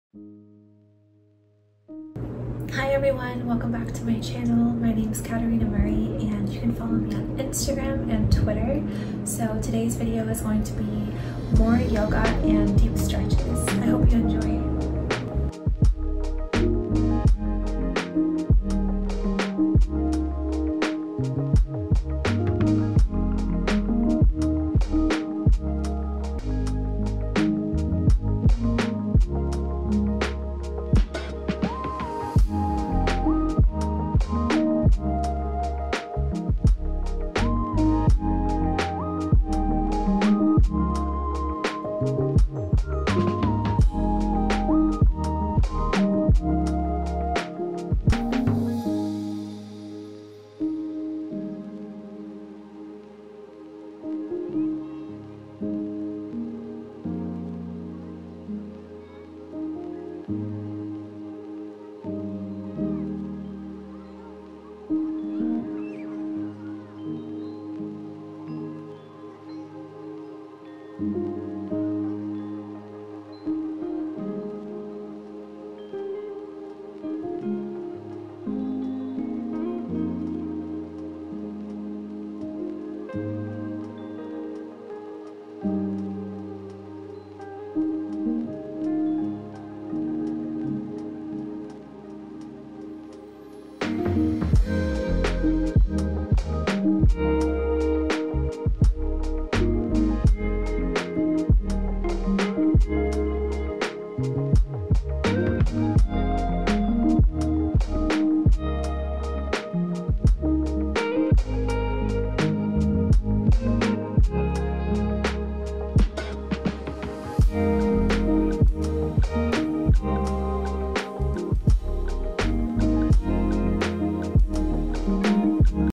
♡「Yoga for Flexibility & Strength 」♡ Stretching Routine + Cute Workout Outfit [4K]
「Katerina Marie Rosaria」
model ✧ cosplayer ✧ elf princess

Join me in this relaxing yet powerful yoga and stretching routine designed to boost flexibility, strength, and mindfulness. Whether you're a beginner or a yoga enthusiast, these poses will help you unwind, improve your posture, and feel more energized throughout the day. Plus, I'll be rocking some of my favorite cute workout outfits to keep things fun and stylish! 🌸

I'm a beginner with yoga and am still working on my flexibility. Let's stretch together and live our best, healthiest lives! ♡